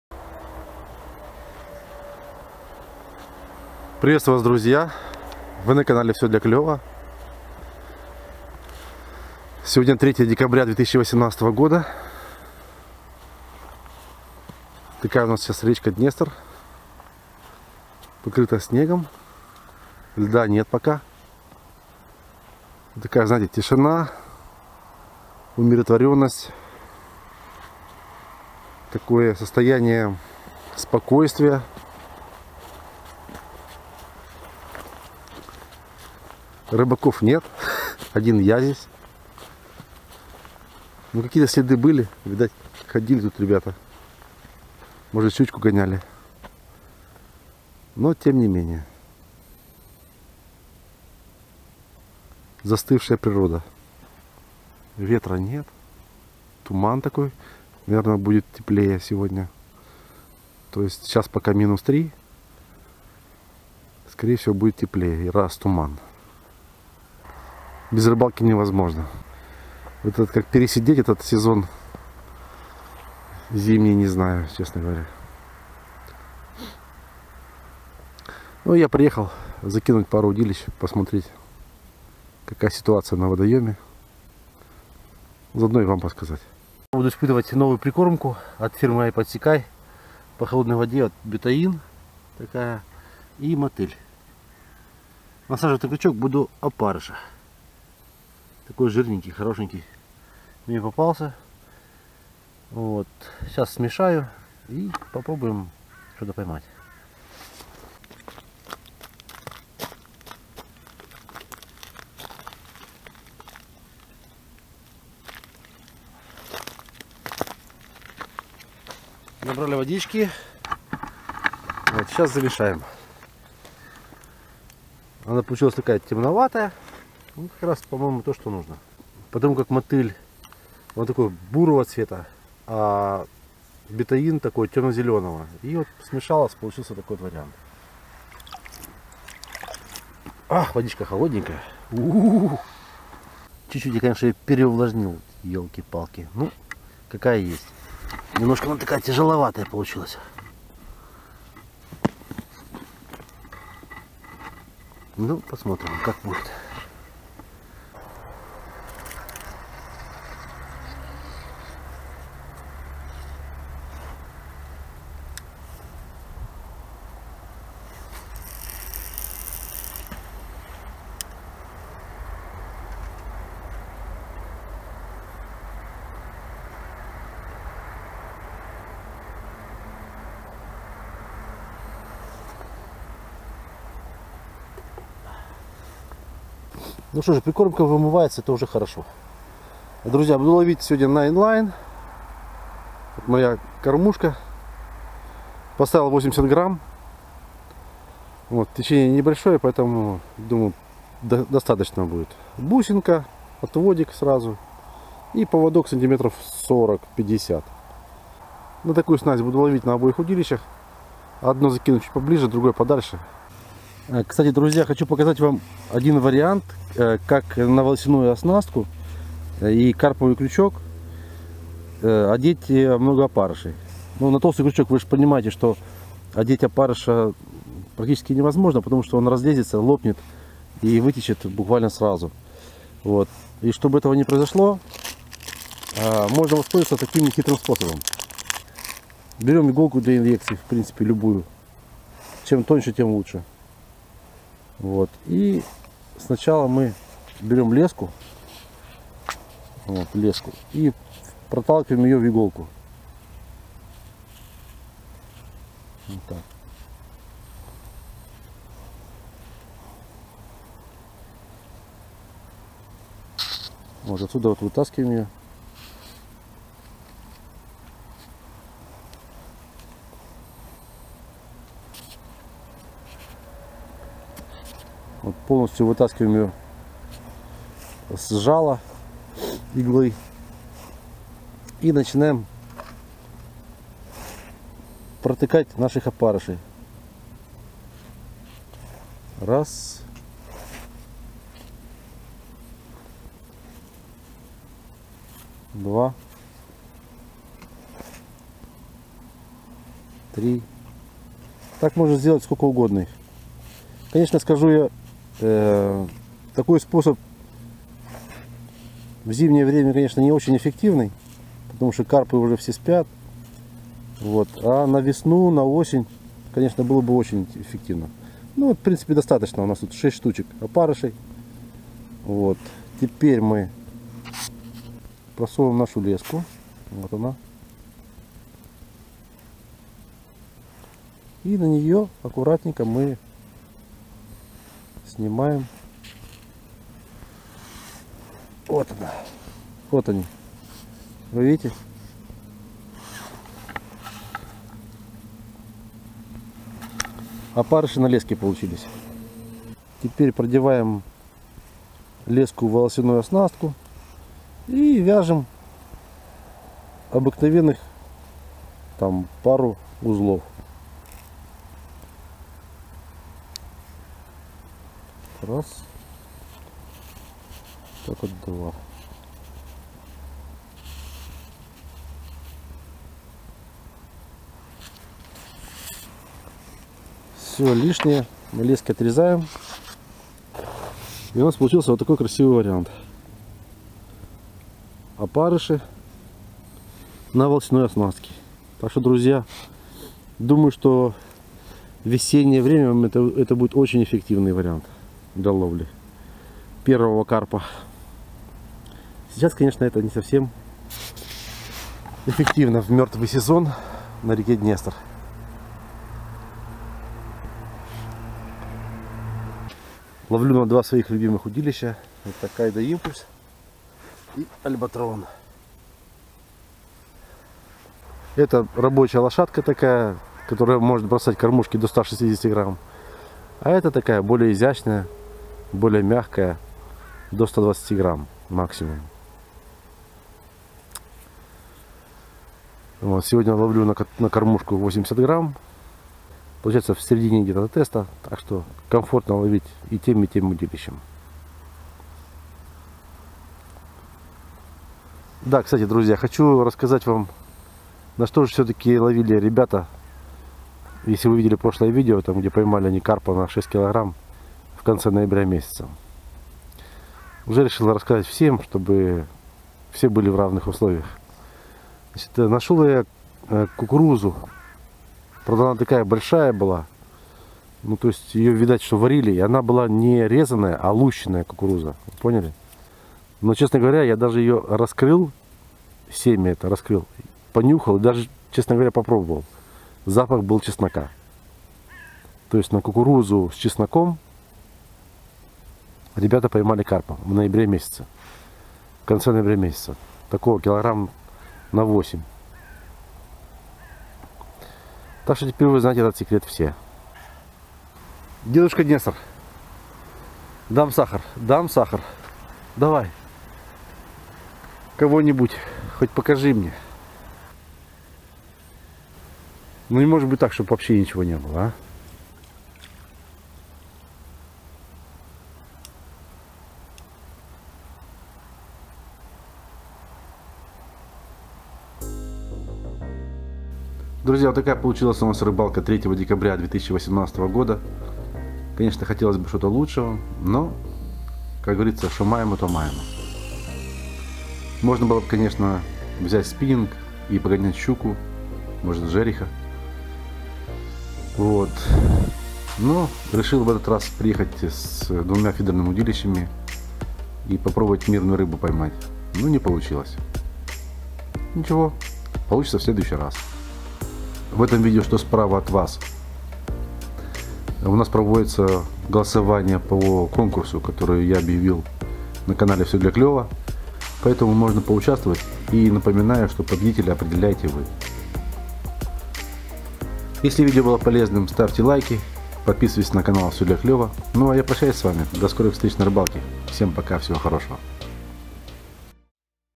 Опарыш на волос/ Рыбалка в начале декабря, Днестр, Маяки.
Как наживить опарышей на волосяную оснастку? Интернет-магазин kleva.com.ua подскажет Вам нюансы. Днестр - щедрая река в которой ещё есть чистая, вкусная, сладкая рыба.  Ловите с нами, и клев не заставит себя долго ждать.
- Kaida impulse-2, фидерное удилище 3,6 м.
Kaida Albatron, фидерное удилище 3,0 м.
- Зимняя прикормка Ай Подсекай «Бетаин»
- Зимняя прикормка Ай Подсекай «Мотыль»
- Кормушки «Ай подсекай», с грунтозацепами 80 грамм.
- Kормушки для крупной фракции «Патап», 100 гр.